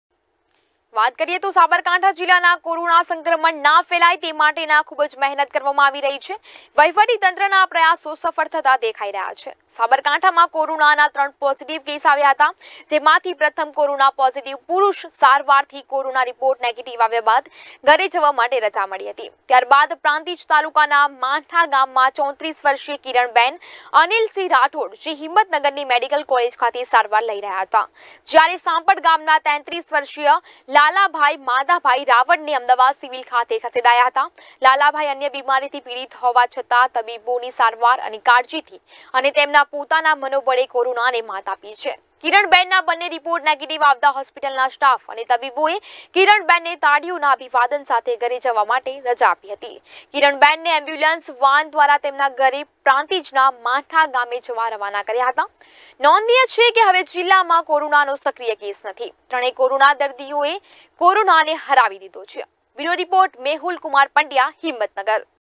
कोरोना को गुडबाय कर दियासाबरकांठा जिल्ला ने तीनो पोजिटिव केश में सारवार के बाद हॉस्पिटल से मुक्ति
साबरकांठा जिल्ले में तीन केश कोरोना पॉजिटिव आये थे तो प्रशास्ग्न ओर डॉक्टरों ओर परिचारिको की कड़ी मेहनत और हौसला बढ़ाने के बाद तीनों केश नेगेटिव हो कर बिल्कुल स्वस्थ पाने के बाद इनको हॉस्पिटल से तालियो की गडगड़ात के साथ स्वस्थ  निरोगी रहे इसी कामनाओ के साथ विदाई दी अब साबरकांठा में आज के दिन कोरोना का कोई केश नही है फिरहाल ओर अब कभी कोइ ऐसा नया केश ना आये यहा ओर पूरे देश मे कंटोल हो जाय ऐसी इस्वर से प्रार्थना करते है , vr live गुजराती न्यूज on (GTPL 274 ) ओर मीडिया ग्रुप पत्रकार मित्रो के सहयोग के साथ मेहुलकुमार पंड्या , जय भारत ।